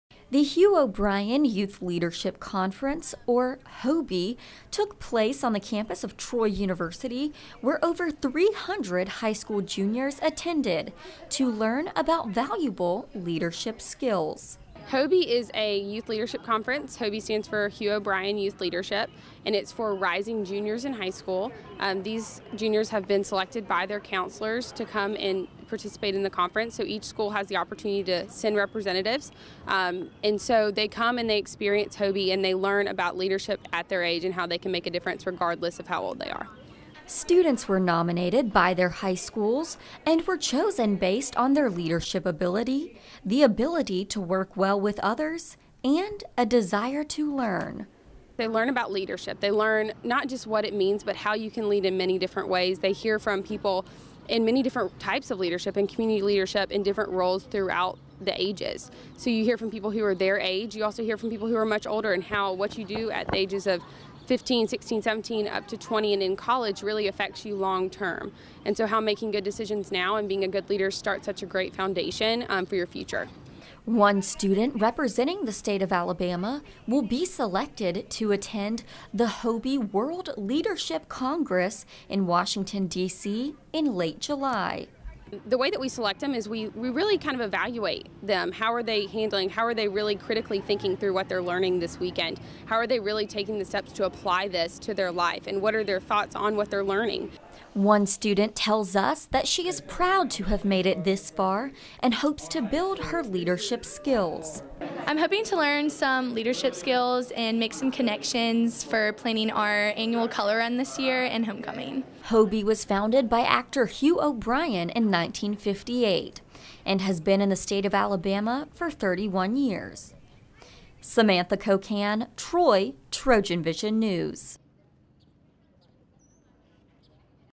Hugh O'Brian Youth Leadership Seminar
Troy University was home to over three hundred high school students this past weekend as the students visited the campus to learn lessons in leadership.  Samantha Kocan has the story.